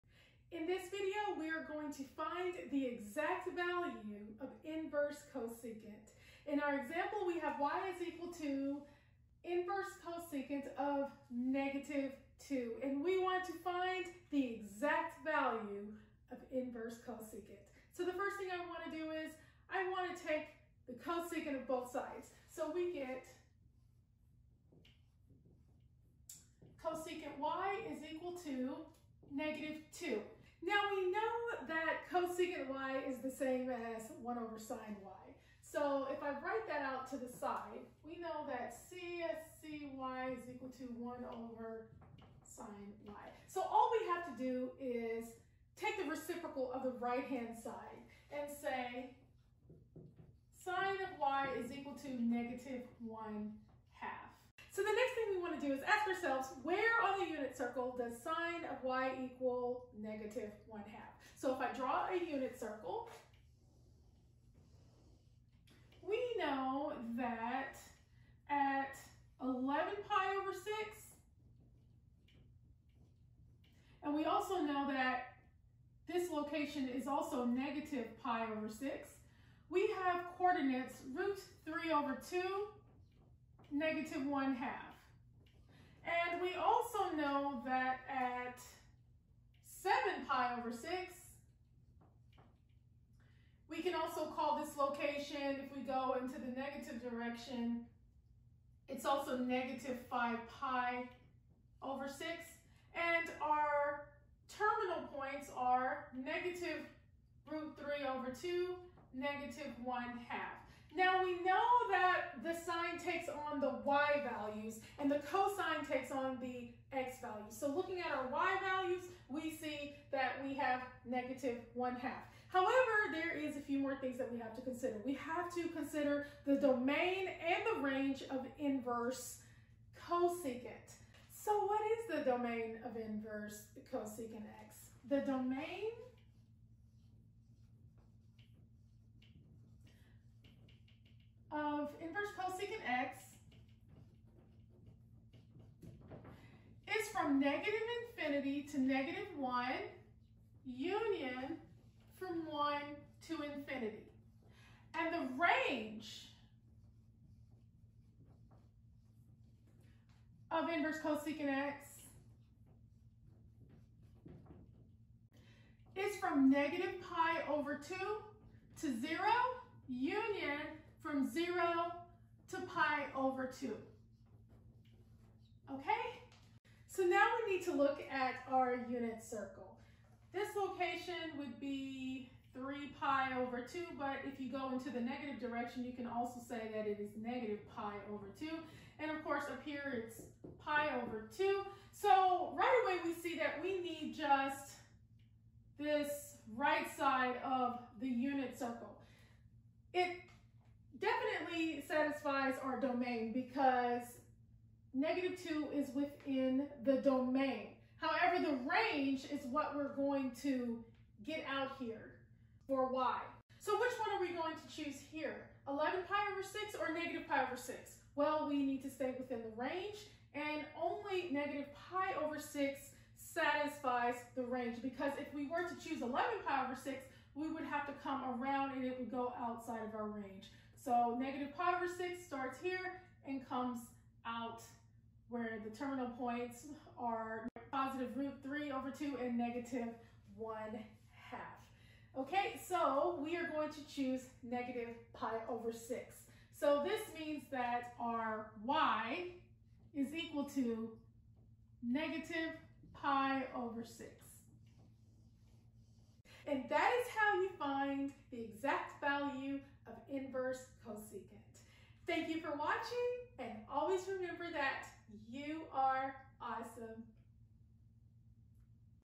Inverse Cosecant (ArcCosecant) | How to Find the Exact Value | (MathAngel369)
Inverse Cosecant | How to Find the Exact Value

In this discussion, we are going to learn how to evaluate inverse cosecant.

Key things to consider:

🔹Observing the domain
🔹Converting csc(y) to sin(y)
🔹Observing the Unit Circle
🔹Observing the Range
🔹Choosing the Value

◻️◻️◻️Concepts◻️◻️◻️

When working with inverse trig functions, in particularly, inverse cosecant, you will have an equation in the form

y = csc^-1(x).

🔷Observing the Domain

Now the domain of  csc^-1(x) is the set of all x such that csc^-1(x) is defined.

In other words, you can think of the domain as all your possible “input” values.

Note that the domain of  csc^-1(x) is

(-inf,-1] U [1,inf)

Thus, we check to see if the x-value satisfies the domain.

If the x-value does not satisfy the domain, we simply say that there is no solution.

If the x-value is within the domain, we then take the cosecant of both sides to get

csc(y) =x

Now it is much easier to evaluate sin(y) instead of csc(y).

Thus, we use the fact that

csc(y) =1/sin(y) or sin(y) =1/csc(y)

Hence, csc(y) =x can be rewritten as

sin(y) = 1/x

Looking at the unit circle, you will see coordinates in the form,  (a,b).

Remember that sine take takes the “b” coordinate.

We then search for all the values of y such that

sin(y) = 1/x,

where 1/x is in the “b” location.

Once we find all the values for y, we consider the range of  csc^-1(x).

🔷Observing the Range

While the domain is your possible input values, the range is your possible output values.

The range of csc^-1(x) is

[-pi/2,0) U (0,pi/2],

which is the right region of the unit circle, but not including 0.

🔷Choosing the Value

You may find multiple values for y; however, we must choose the value for y that satisfies the range.

◻️◻️◻️Example in the Video◻️◻️◻️

Let us say you are given,

y = csc^-1(-2).

Here are the steps:

🔷Observing the Domain

 (-inf,-1] U [1,inf).

Since x=-2 in our example, we know that it satisfies the domain.

Since our x-value is within the domain of csc^-1(x), we then take the cosecant of both sides to get

csc(y) = -2.

Again, it is much easier to evaluate sin(y) instead of csc(y).

So we use the fact that

Thus, csc(y) = -2 can be rewritten as

sin(y) = -1/2

Looking at the unit circle, we search for all the values of y such that  sin(y) = -1/2.

I like to ask myself, “Where on the unit circle does sin(y) = -1/2?

On the unit circle, we have coordinates
 (a,b) =  (sqrt(3)/2, -1/2) at the following angles:

11pi/6 (If we go in the positive direction)

-pi/6 (If we go in the negative direction)

7pi/6 (If we go in the positive direction)

-5pi/6 (If we go in the negative direction)

Remember, we observe the “b” coordinate when working with sine.

Thus, when:

y = 11pi/6, -pi/6, 7pi/6, and -5pi/6

we get:

sin(11pi/6)=-1/2
sin(-pi/6)=-1/2
sin(7pi/6)=-1/2
sin(-5pi/6)=-1/2

🔷Observing the Range

Now, we cannot choose any value for y in our ultimate answer. We must consider the range of  ccs^-1(x).

[-pi/2 , 0) U (0 , pi/2],

which is the right region of the unit circle, but not including zero.

🔷Choosing the Value

Remember that we must be sure that our value for y is within the range!

We see that the only value within the range is -pi/6.

Thus, we choose -pi/6.

Hence,

sin(-pi/6) =-1/2,

which means that

csc(-pi/6)=-2,

which means that

-pi/6 = csc^-1(-2),

and we say that

y=-pi/6.

We are done!

➕MathAngel369➕